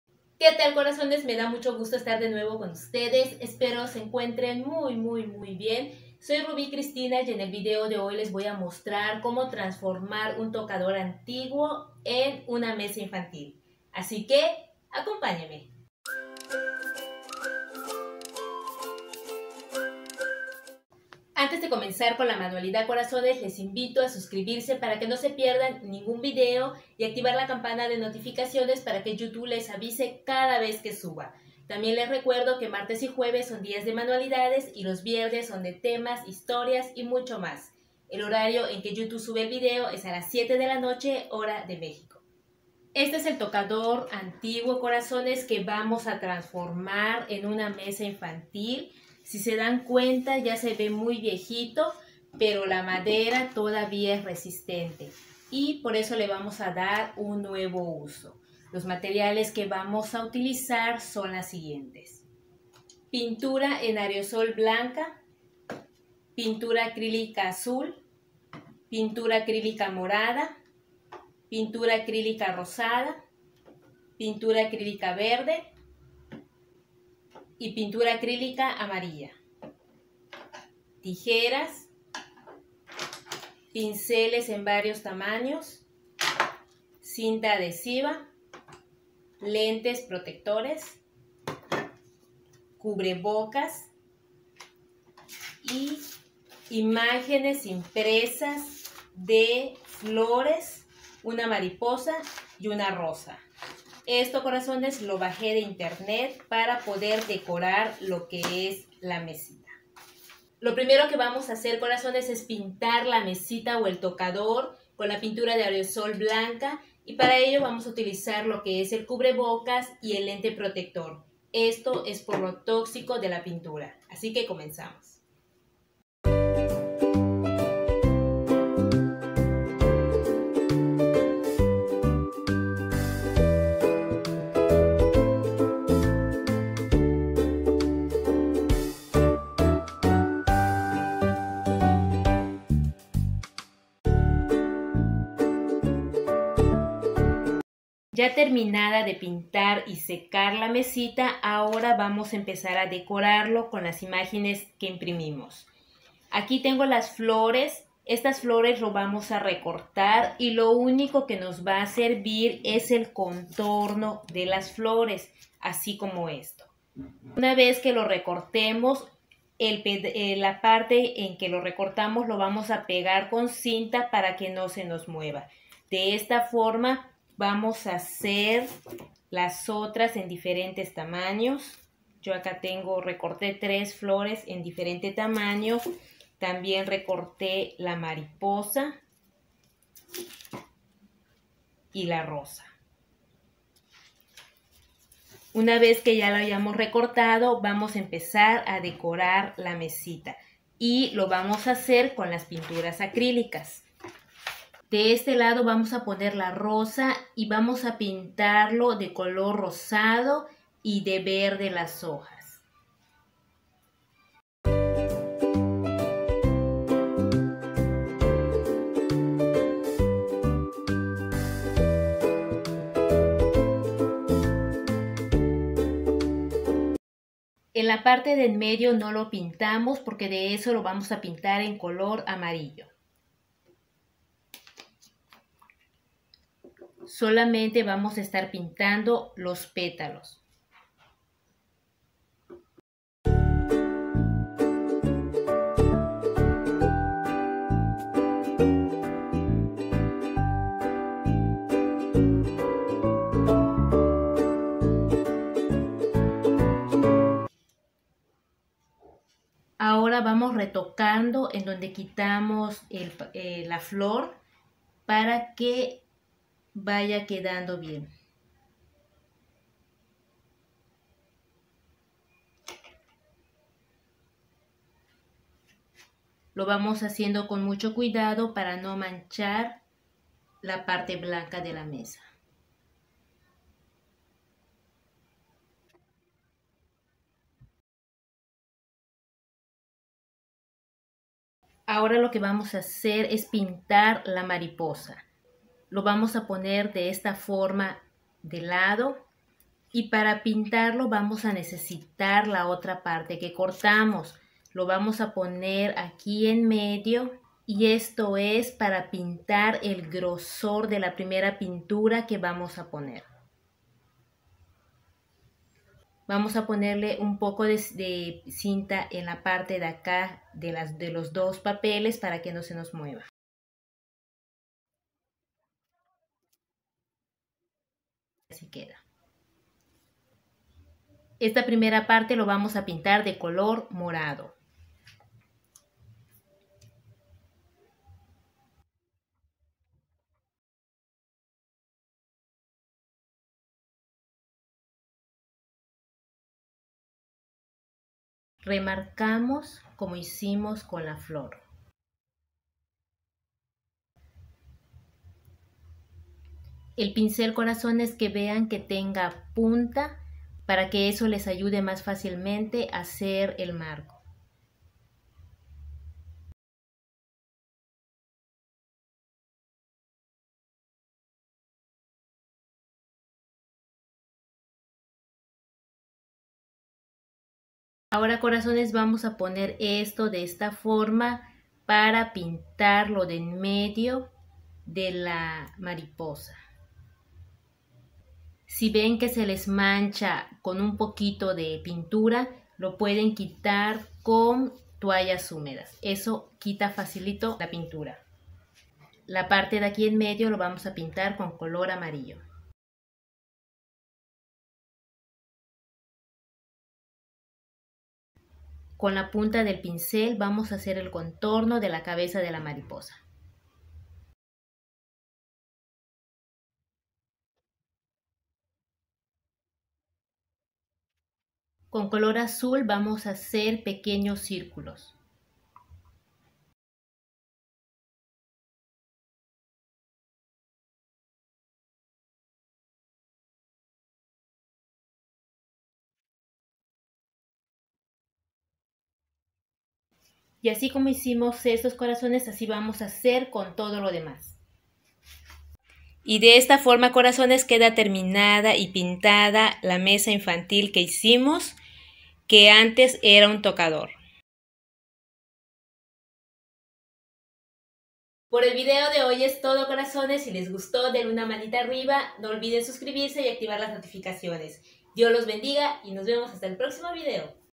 TRANFORMANDO UN TOCADOR ANTIGUO EN UNA MESA INFANTIL
Hola corazones en este video les muestro como darle un nuevo uso a un tocador antiguo que tenia en casa convirtiendola en una mesa infantil. Si tu tienes alguna mesa como esta y no sabes que hacer aqui te doy una idea para que la reutilices y le des un buen uso sobre todo arreglarla para que tus hijos tengan en donde hacer la tarea, les encantará.

Seamos amigos en Instagram y Facebook.